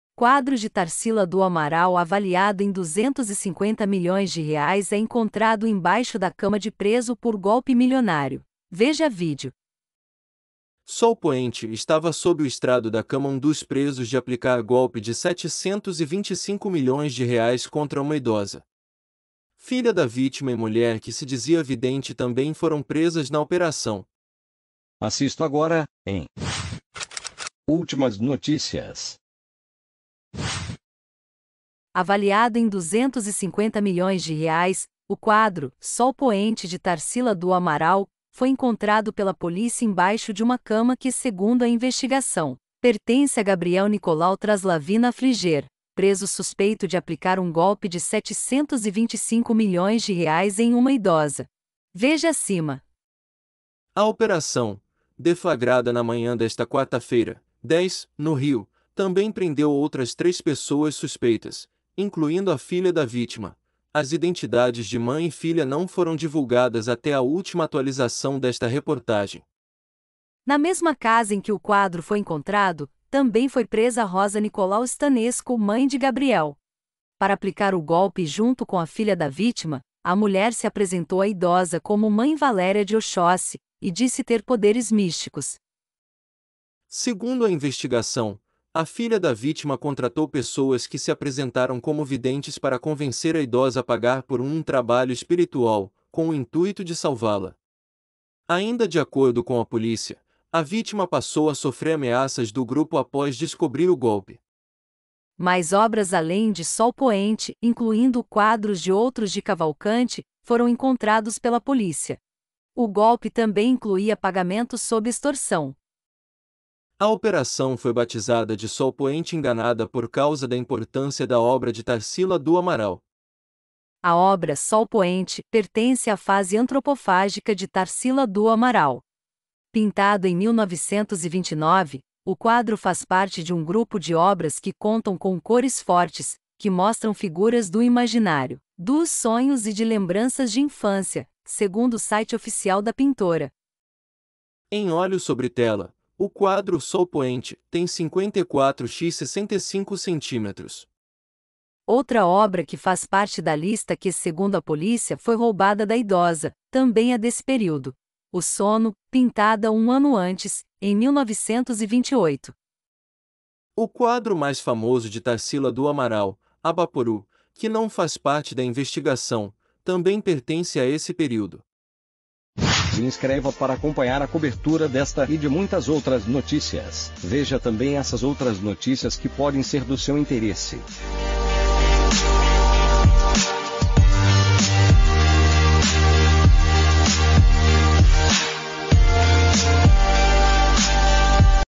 🔴 QUADRO DE TARSILA DO AMARAL AVALIADO EM R$ 250 MILHÕES É ENCONTRADO EMBAIXO DA CAMA DE PRESO POR
'Sol Poente' estava sob o estrado da cama um dos presos de aplicar golpe de R$ 725 milhões contra uma idosa. Filha da vítima e mulher que se dizia vidente também foram presas na operação.

Quadro de Tarsila do Amaral avaliado em R$ 250 milhões é encontrado embaixo da cama de preso por golpe milionário; veja vídeo

🔴 Você curte um FUTEBOL? Sabia que você pode LUCRAR ALTO e ainda se divertir torcendo pro seu time?

Acessado em: 10 de agosto de 2022 às 14:30